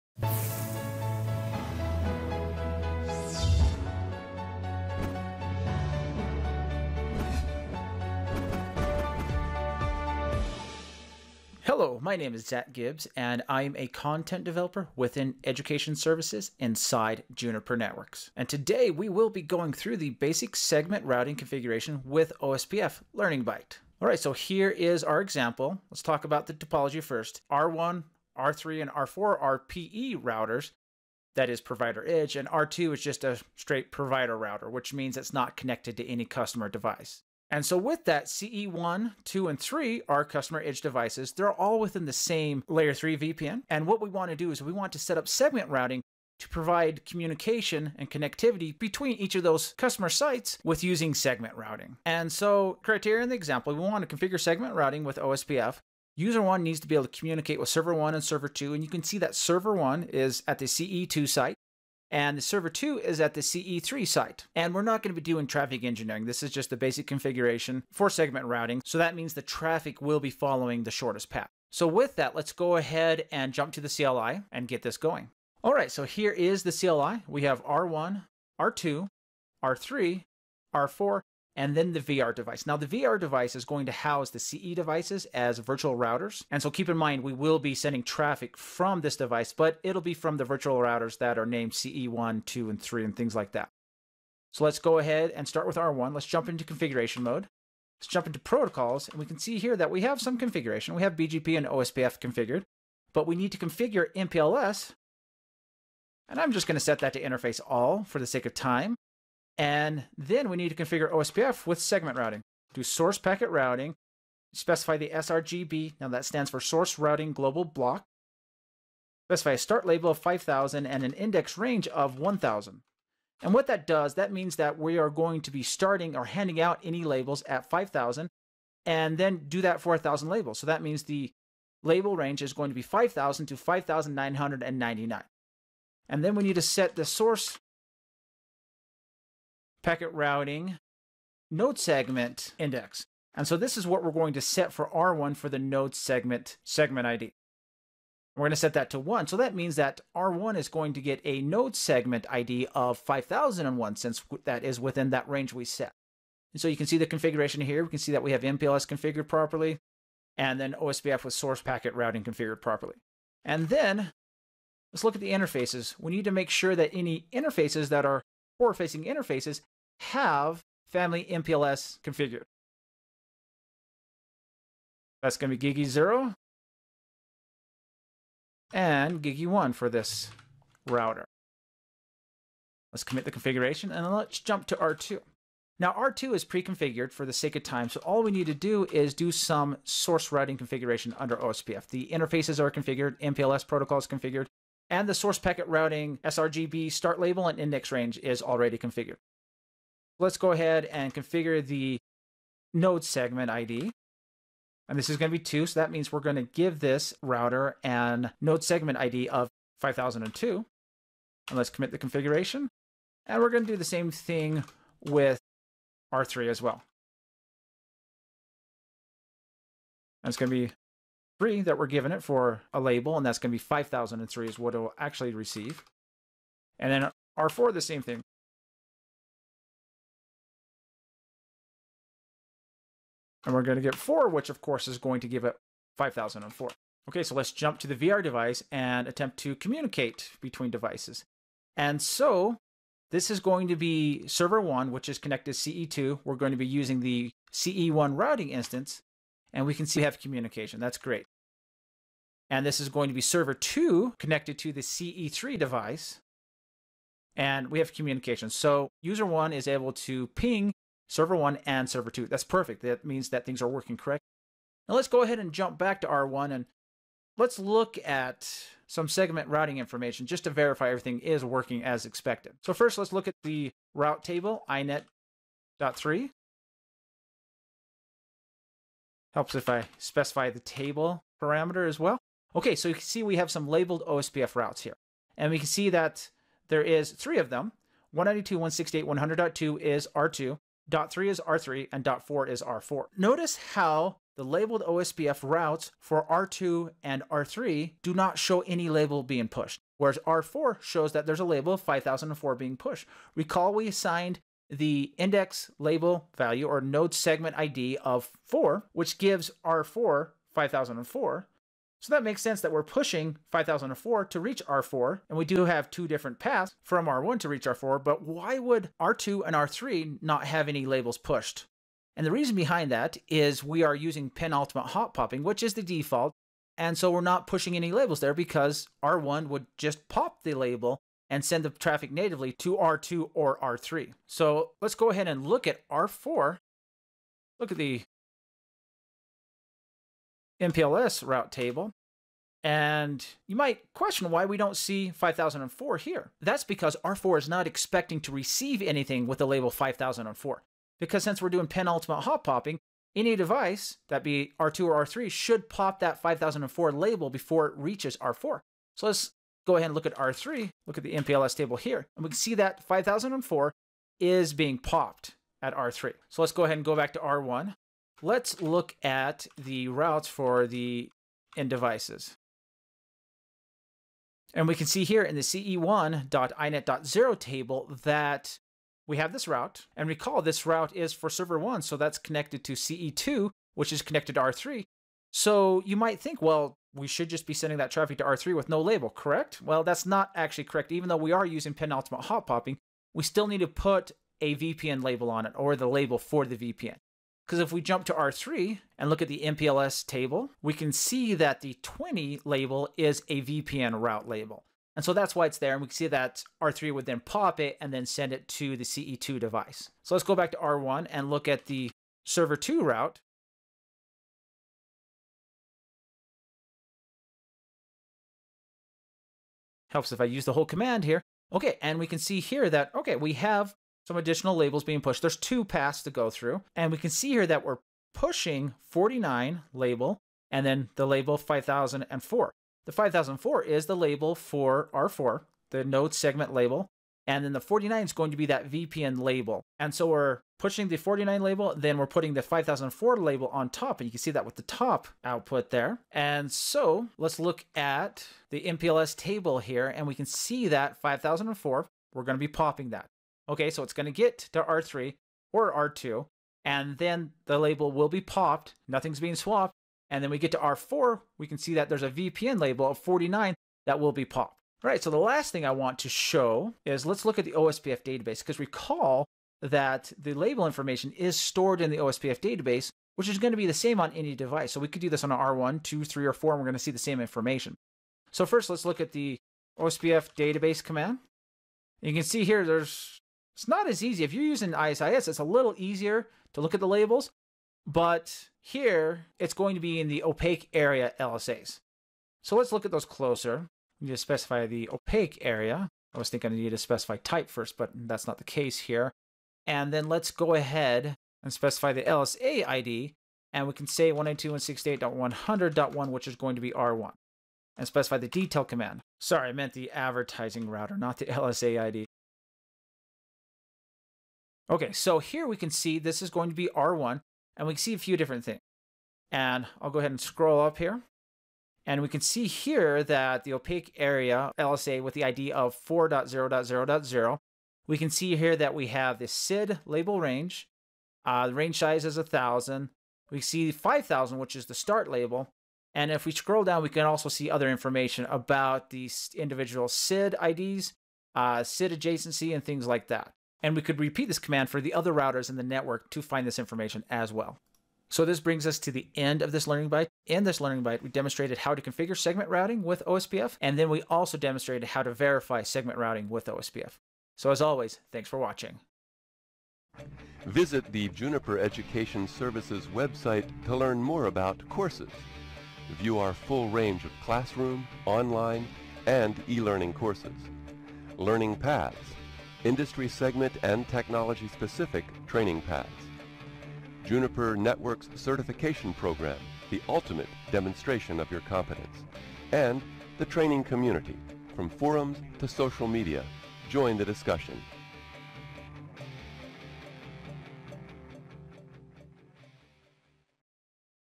Basic Segment Routing Configuration with OSPF
The Basic Segment Routing Configuration with OSPF Learning Byte covers how to configure and monitor segment routing using OSPF. This Learning Byte is most appropriate for users who are new to working with segment routing.
• Presenter: Zach Gibbs / Content Developer
• Relevant to OS Releases: Junos 20.1R1 or later
• Relevant to Juniper Platforms: MX Series, vMX Series, PTX Series, and T Series devices

• On-Demand Training: Take a hands-on course…now!